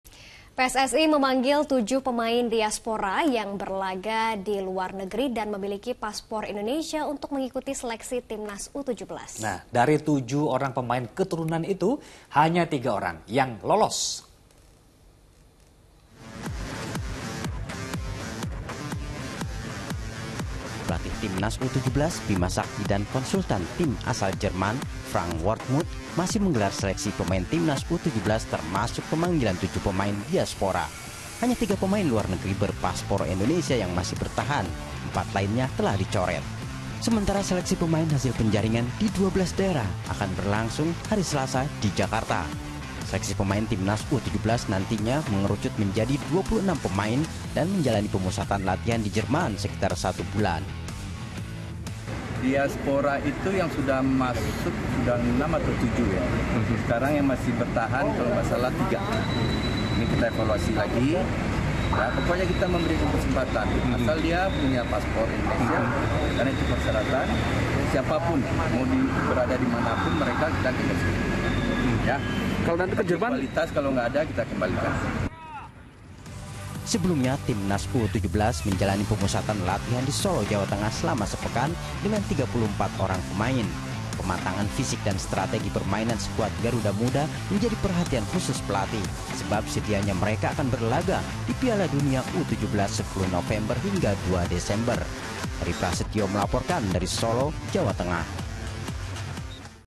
PSSI Akan Seleksi Pemain Diaspora untuk Gabung di Timnas U-17 | Fokus
PSSI memanggil tujuh pemain Diaspora yang berlaga di luar negeri dan memiliki paspor Indonesia untuk mengikuti seleksi Timnas U-17. Dari tujuh pemain keturunan itu, hanya tiga yang masih bertahan di Timnas U-17.